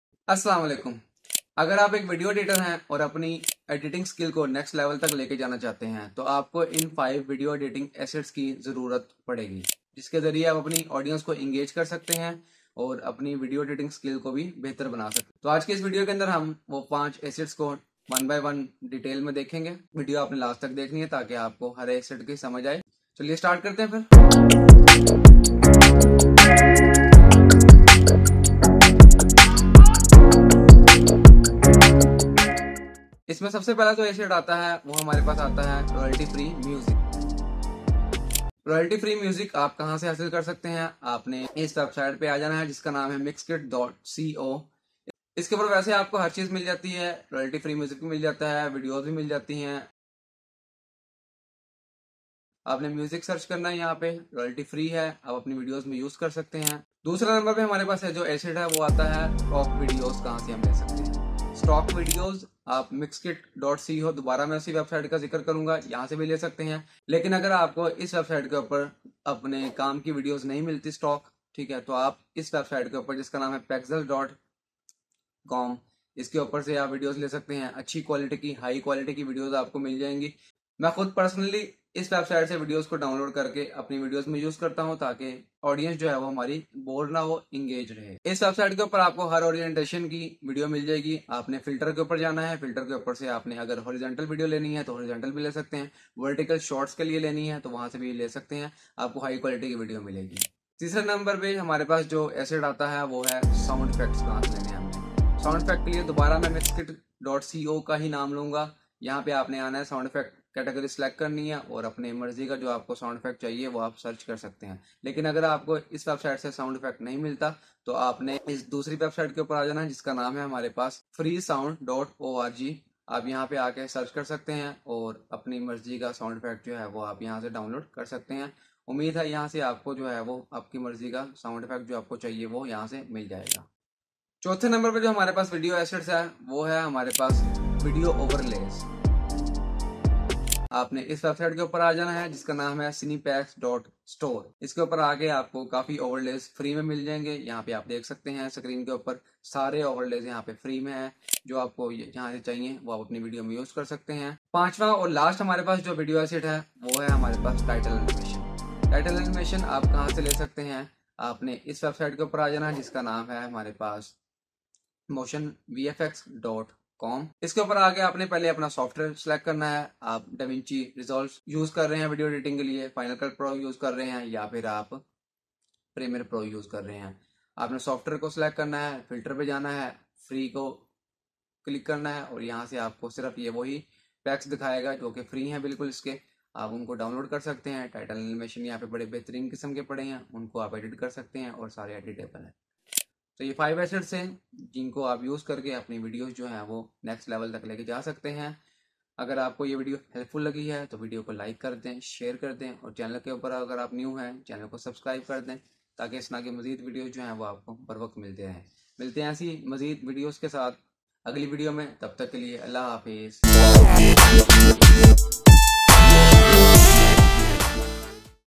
5 Assets for Video Editing You Must Know
Master Video Editing with These 5 Essential Assets

In today's video, we're diving deep into the world of video editing and uncovering the 5 essential assets every video editor must know about. Whether you're a beginner looking to up your editing game or a seasoned pro seeking to enhance your skills, this video is packed with valuable information to help you create stunning videos.

Whether you're creating content for YouTube, social media, or any other platform, these assets will be your secret weapons for producing outstanding videos. Join us as we break down each asset step by step and provide practical tips and recommendations along the way.